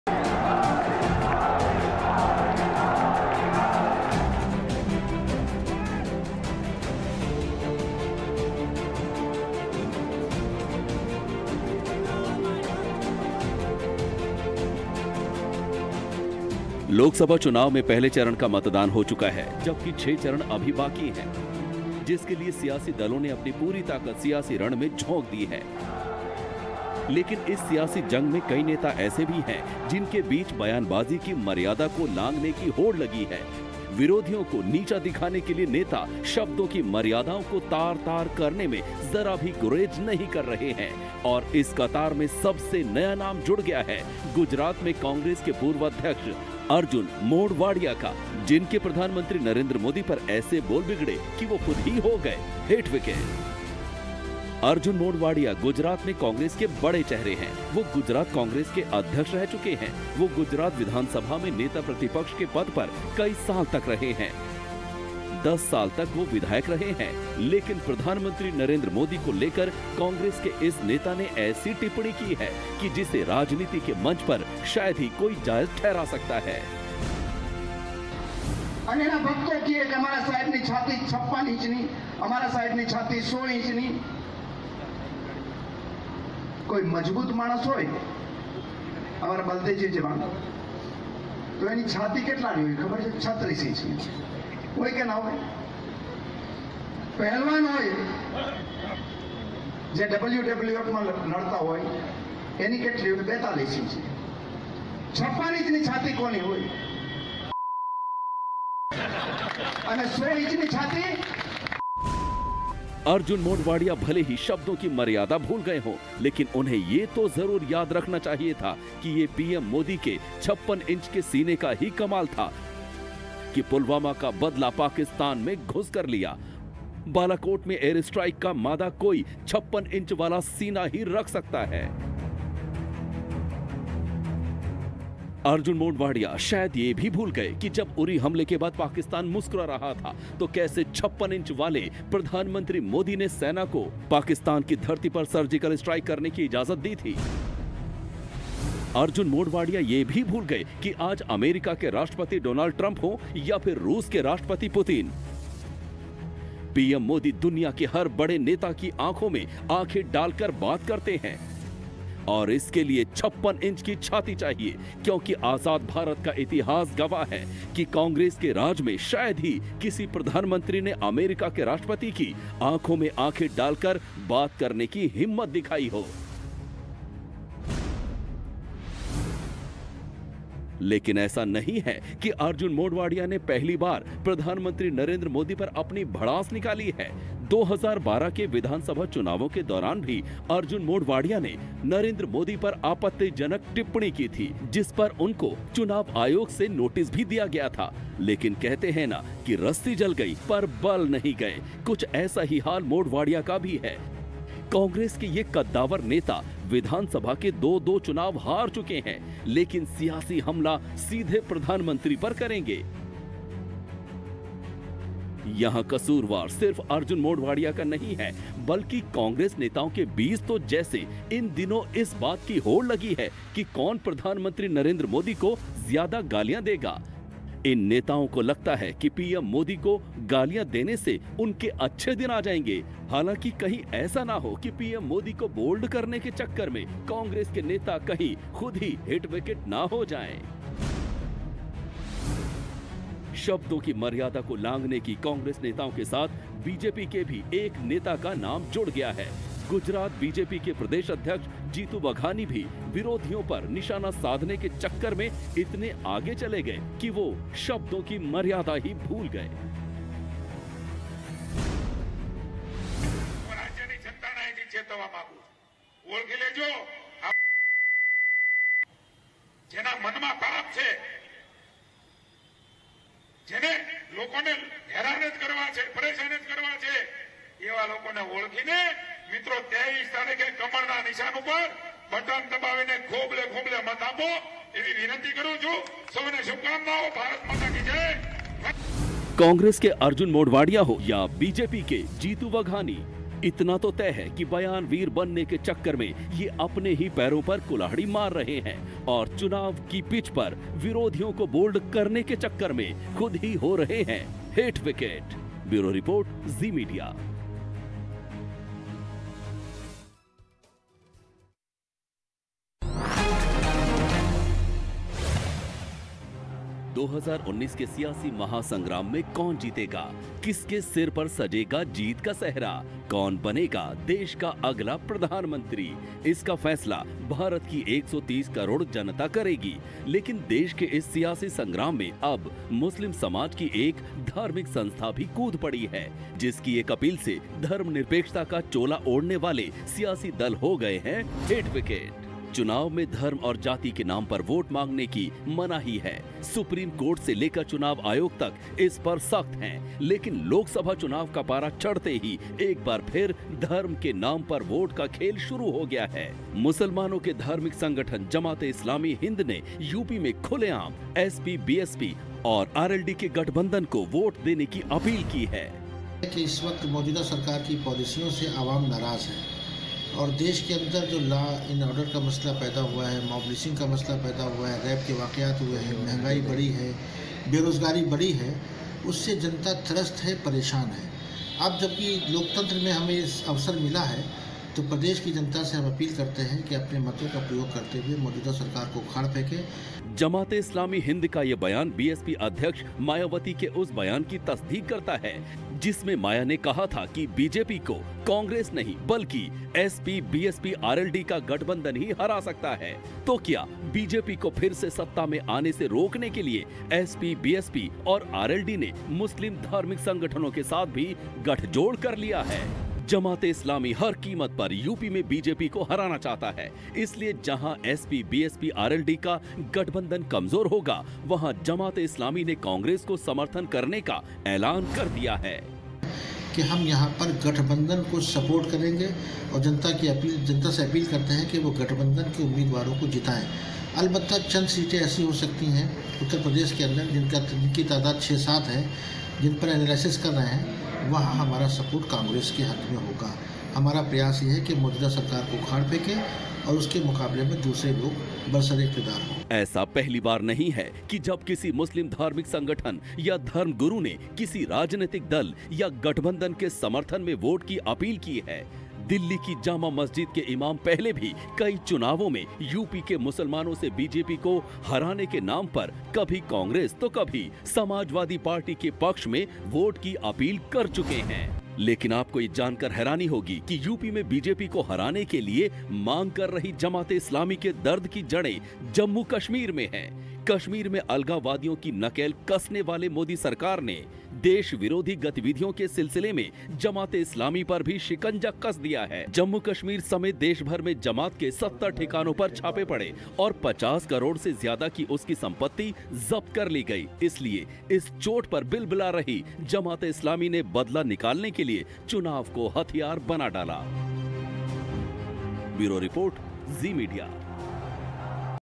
Hit Wicket : चुनावी समर में पीएम मोदी पर सियासतदानों के बिगड़े बोल
चुनावी समर में कई नेताओं की जुबान बेलगाम हो गई है. चुनावी पिच पर नेताओं की जुबान लगातार फिसल रही है.

About Channel